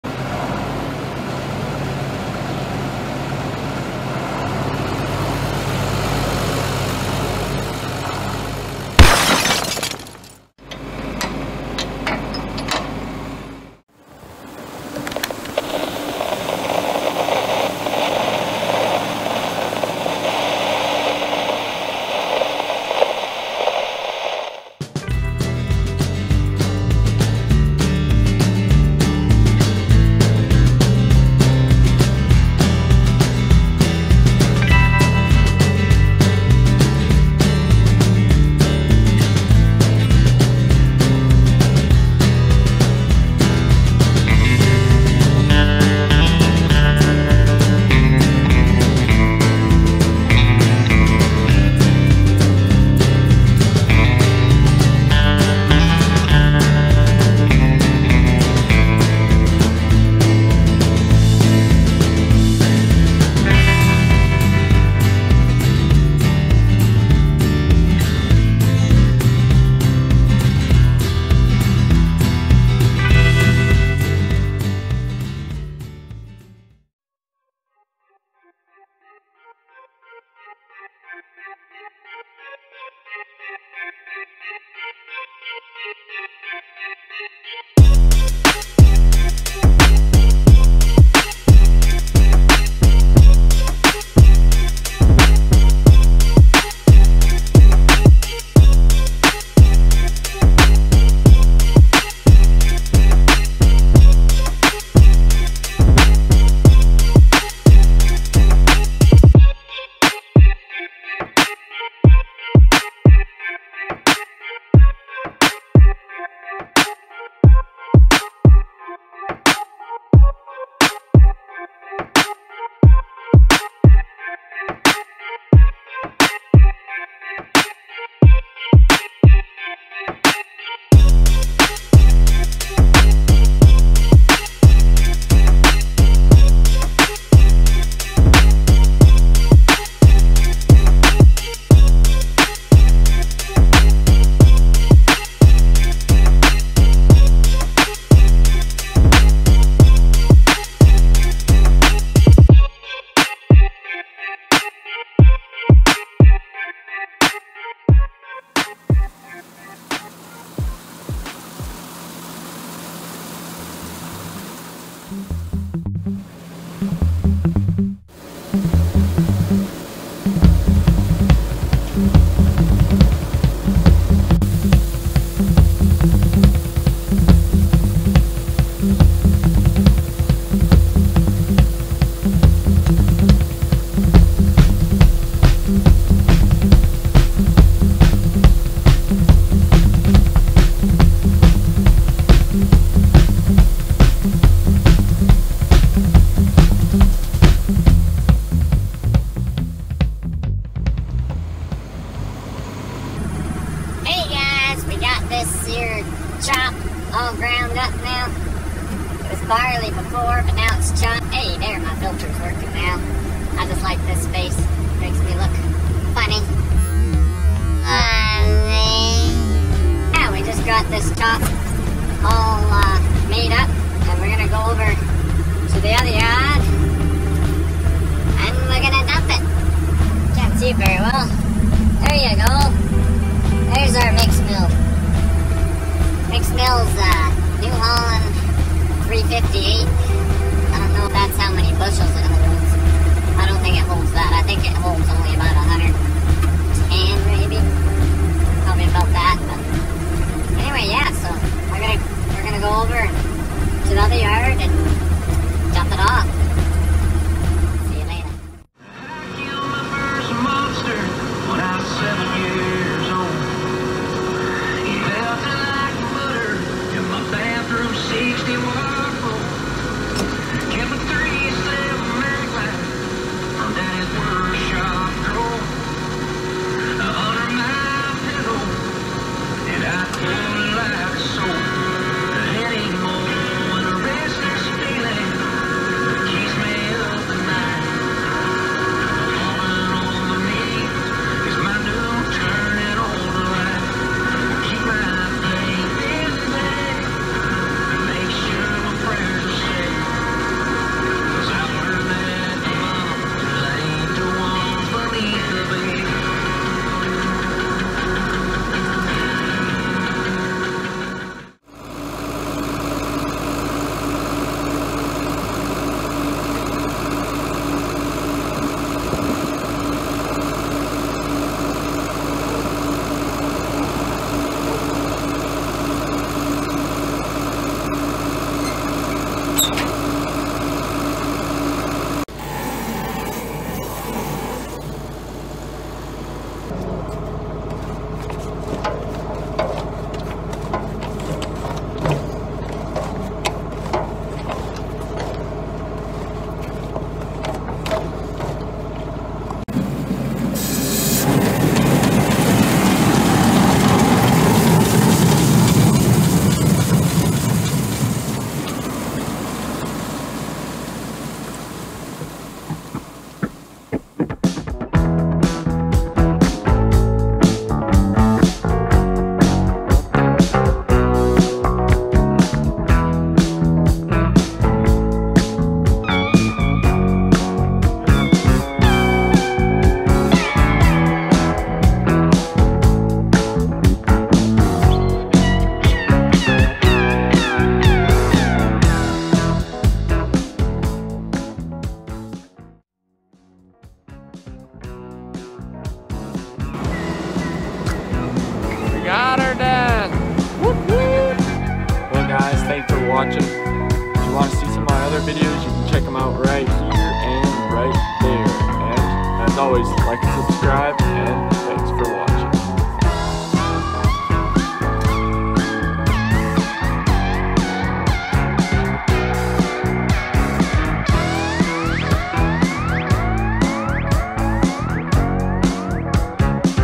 Grinding Barley Today!
Grinding up the Barley for the cows with the good ole' 4430 JD tractor and the New Holland 358 mix mill!
Why do we grind our Barley you might ask? Can't ya just feed er' whole? Well, when the barley is ground up, the cattle can get the nutrients quicker and its also safer for the animals! Then, the grain won't swell in the gut, which could have caused the cows to get a stomach bloat if the barley wasn't ground up.

 Dirt and Barbed Wire out!